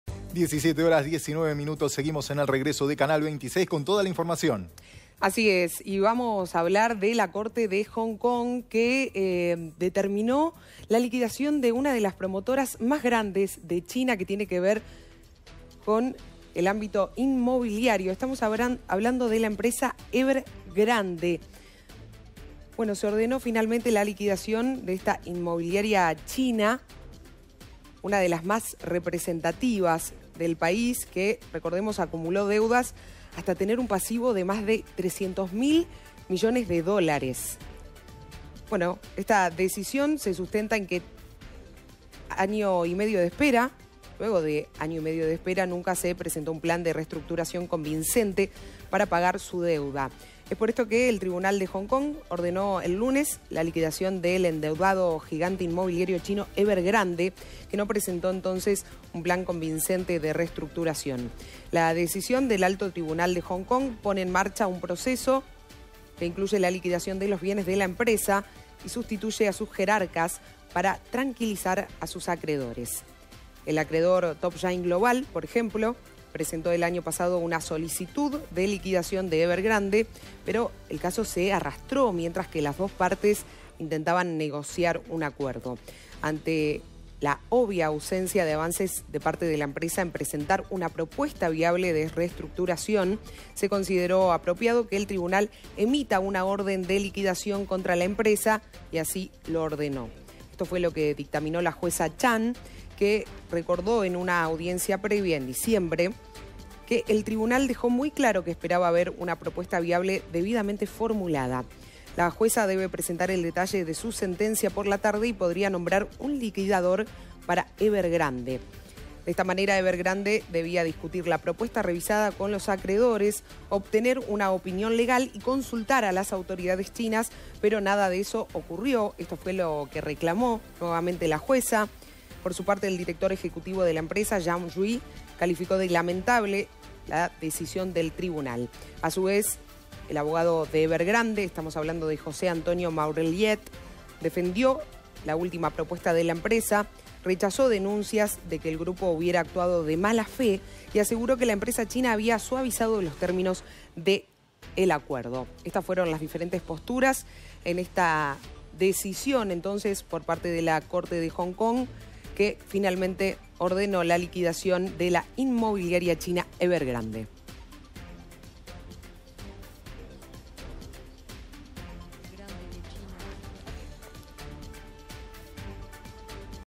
Corte de Hong Kong ordena liquidación de inmobiliaria china Evergrande
👉 La decisión se sustenta en que, tras año y medio de espera, nunca se presentó un plan de reestructuración convincente para pagar su deuda.